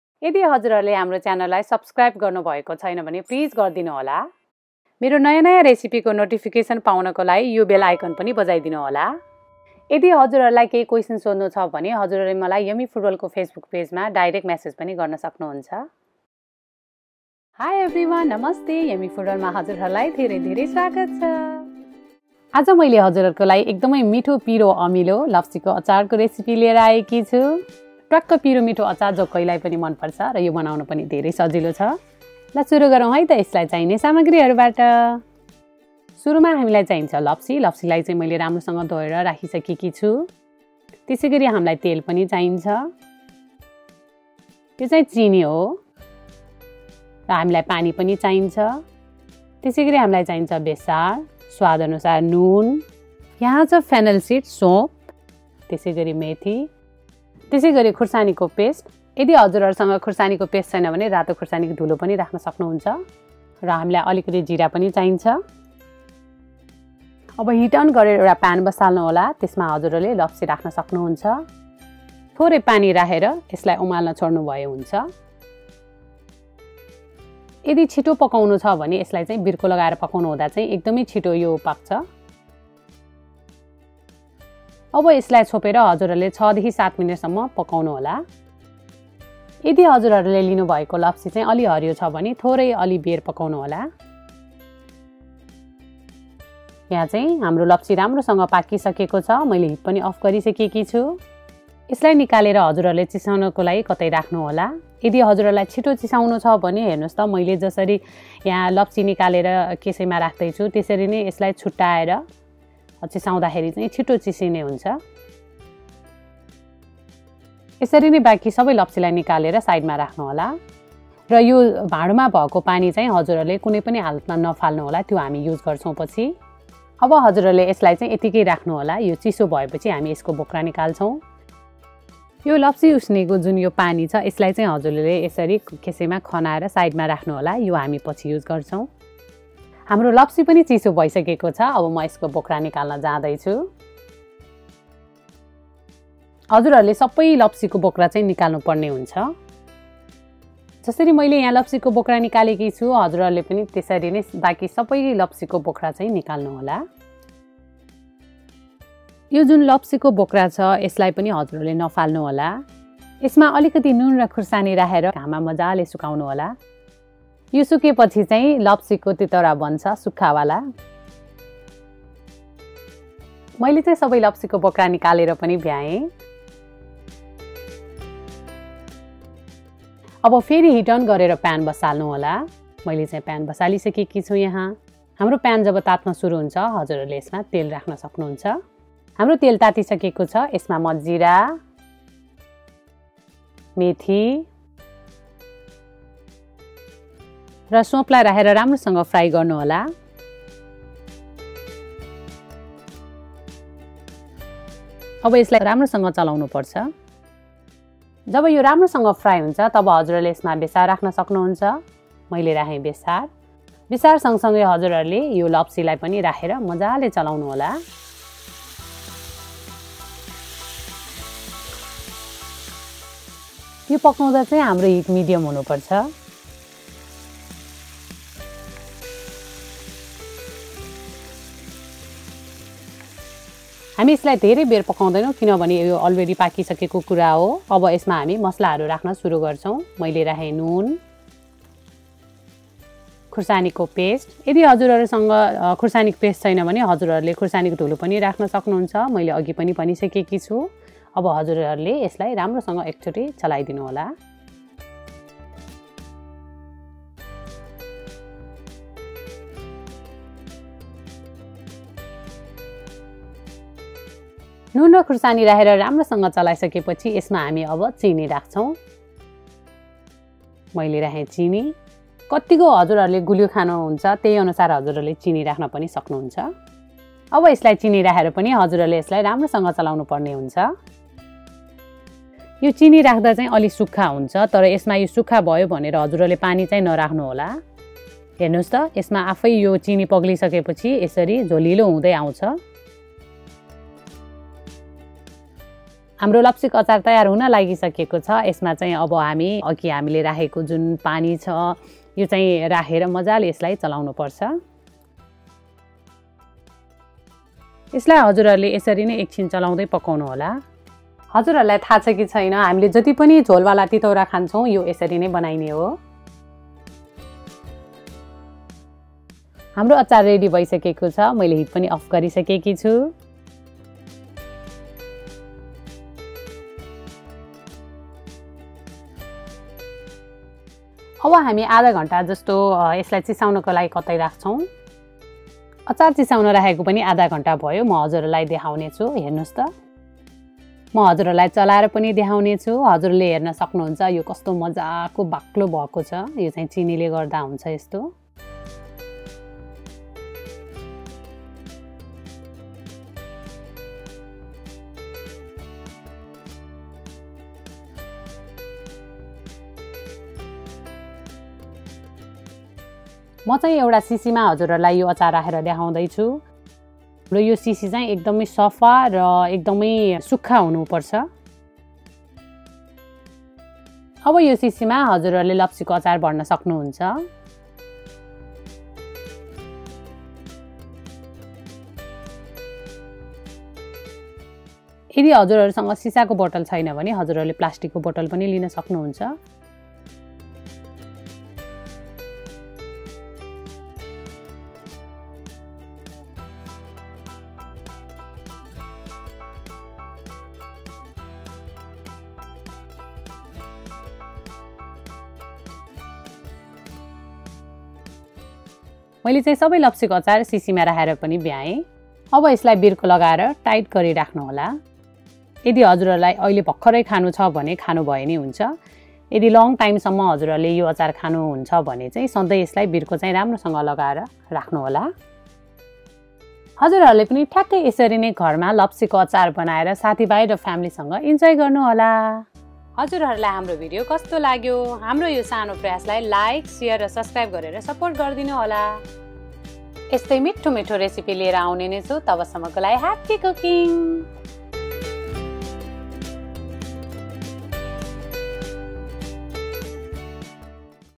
Lapsi ko Achar | How to make Lapsi ko Achar | लप्सीको आचार | Yummy Food World
WELCOME to YUMMY FOOD WORLD |
NEPALI FOOD RECIPE
Coconut Laddu RECIPE IN NEPALI LANGUAGE
NEPALI / NEWARI CUISINE
NEPALI FOOD RECIPE CHANNEL
HOW TO MAKE Lapsi ko Achar

Thanks for stopping by and watching this video.

INGREDIENTS NEEDED TO PREPARE

Lapsi -  500 g
Sugar -  50 g
Cumin Seeds – 1.5 table spoon
Fennel Seeds – 1.5 table spoon
Fenugreek Seeds – 1.5 table spoon
Salt as per your taste or 1 table spoon

Hope you enjoyed my recipe for Coconut Laddu | Lapsi ko Achar (KIDS as well as ADULT LOVING FOOD)

See you soon… Have a Good day & Happy Cooking :-)